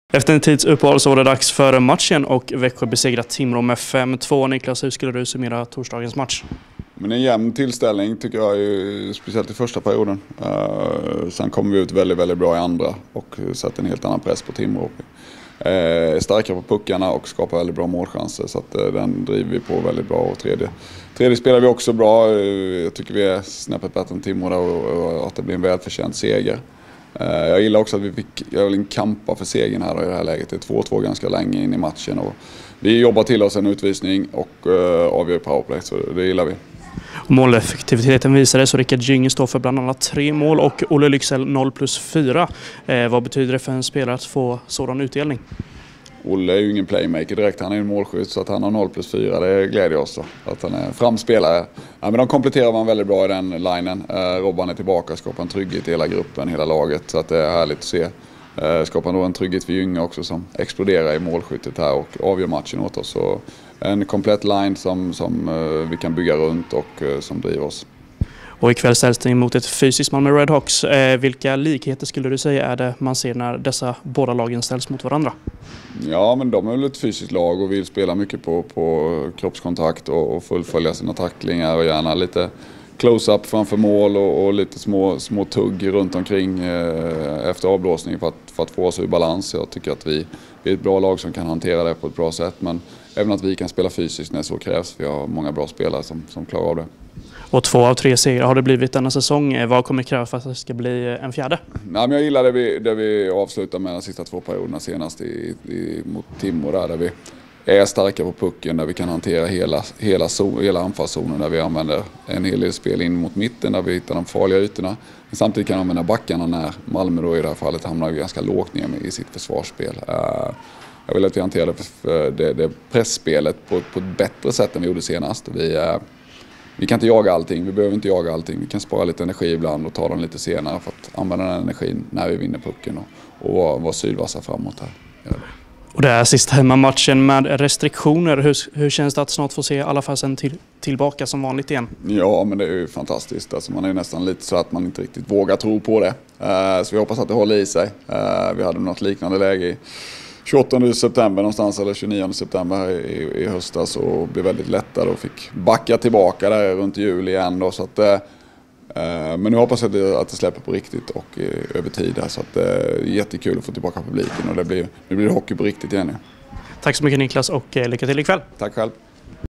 Rahm inför kvällens hemmamatch
Ikväll gästas Växjö Lakers och VIDA Arena av Malmö Redhawks för lördagsmatch med nedsläpp 18:00. Hör assisterande tränare Nicklas Rahm om hur man ska följa upp torsdagens 5-2 seger.

Säkra din plats till kvällens match - Biljetter från 150 kr!